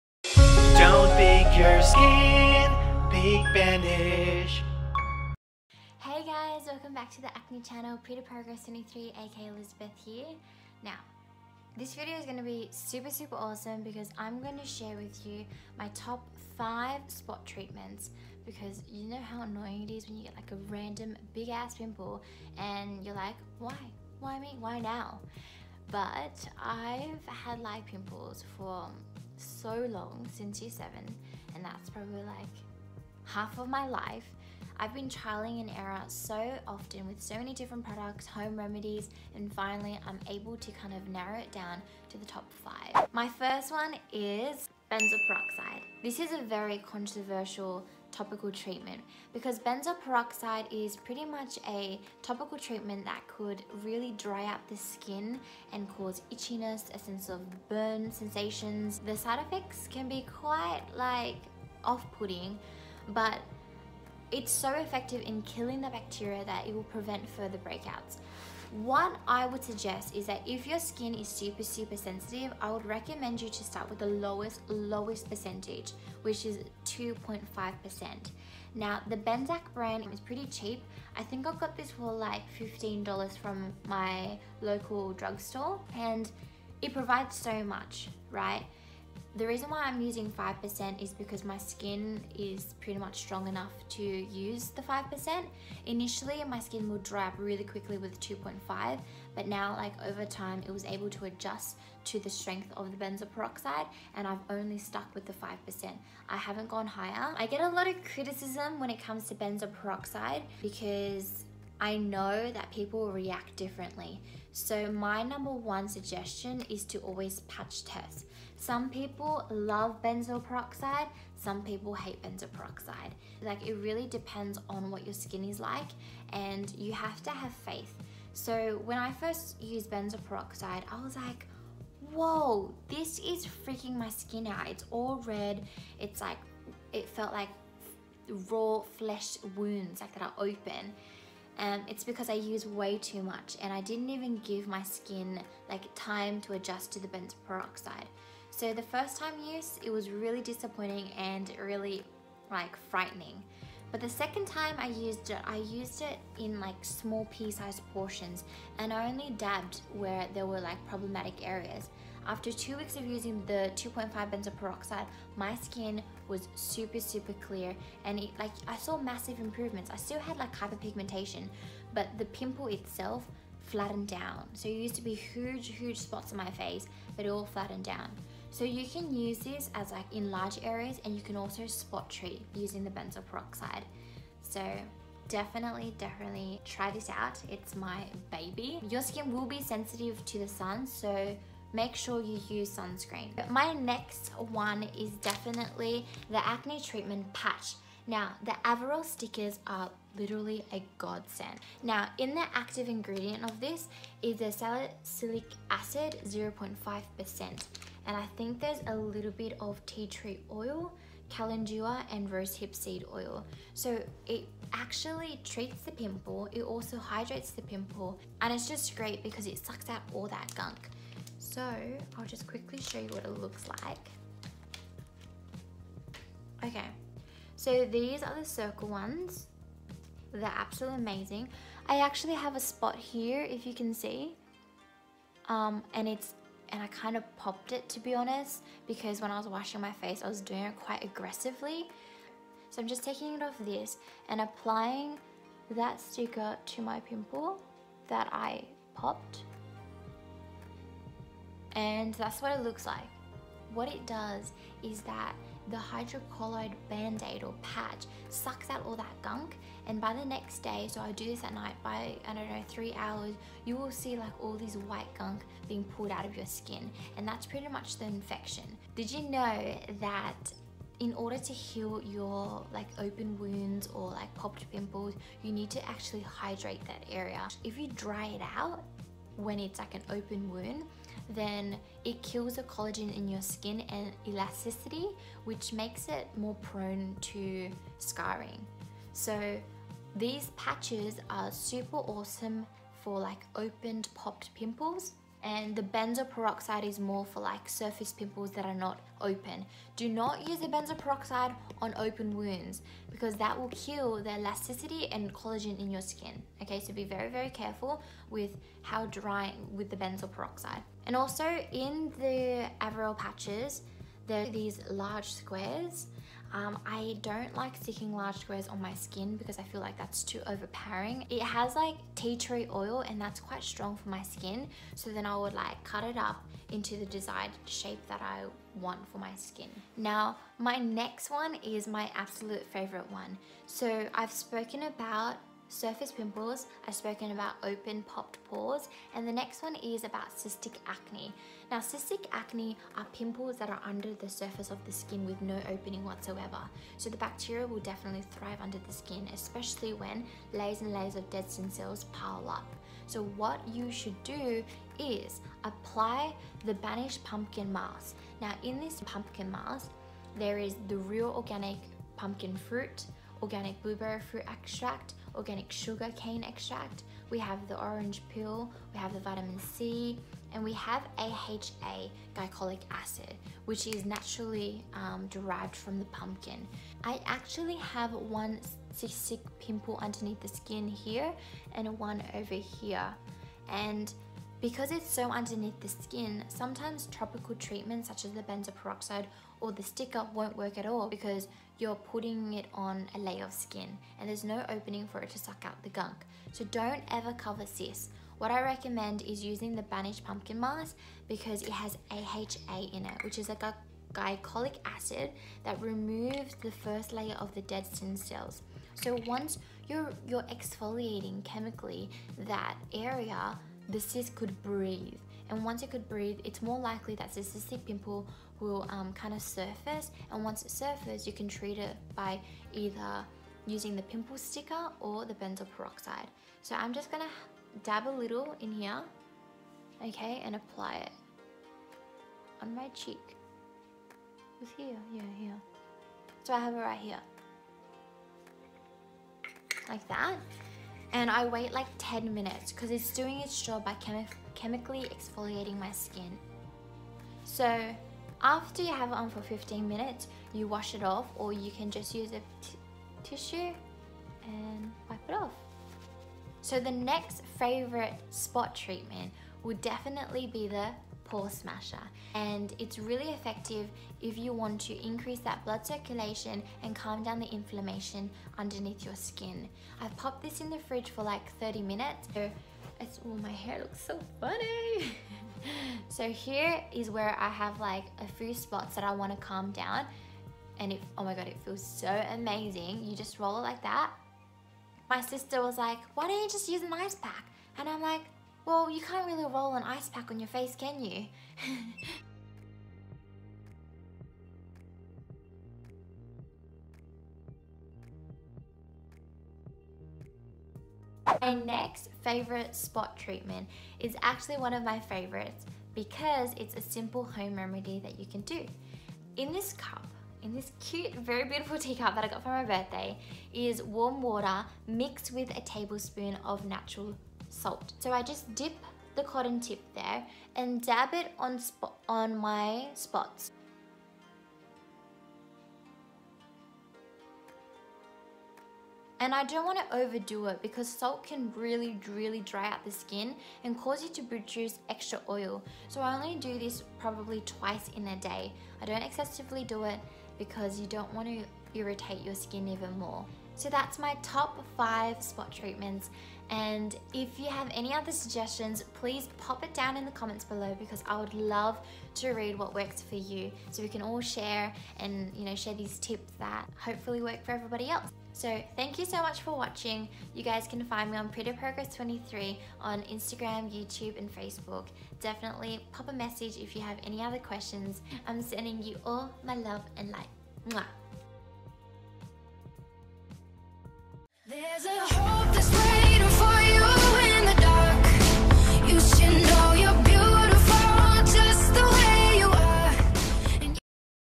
Top 5 Acne Treatments that Work!
Hey, guys! I'm sharing with you my TOP 5, holy grail, tried and tested acne treatments that actually work! I know you're excited about this! 😎

1. Benzoyl Peroxide - I know this is a controversial treatment cause of the effects such as dryness, itchiness, etc. I recommend that if you want to try this, to start with the 2.5% and start with a patch test, definitely!

This works by killing the bacteria on your skin to prevent further breakouts. You can also use it as a spot treatment.

2. Avaralle Acne Treatment Patch - I LOVE THIS! The patch has salicylic acid, tea tree oil, calendula, and rosehip seed oil.

I love this cause you can actually see the gunk that the patch sucked out in just a few hours! These patches are great for open, popped pimples!

This mask can be used as a spot treatment and this is amazing for cystic acne! This mask contains AHA glycolic acid derived from pumpkin! This removes the first layer of dead skin cell and exfoliates your skin chemically!

This is really effective to increase blood circulation and calm down the inflammation underneath your skin - it feels amazing and calming too!

5. DIY Treatment: warm water + natural salt

This is super easy, affordable and effective. This will help dry out your acne, I recommend to use this 1 to 2 times a day max!

GET RID OF ACNE SCARS WITH THE NUMBER ONE AT-HOME MICRONEEDLING SYSTEM, THE BANISH KIT 2.0: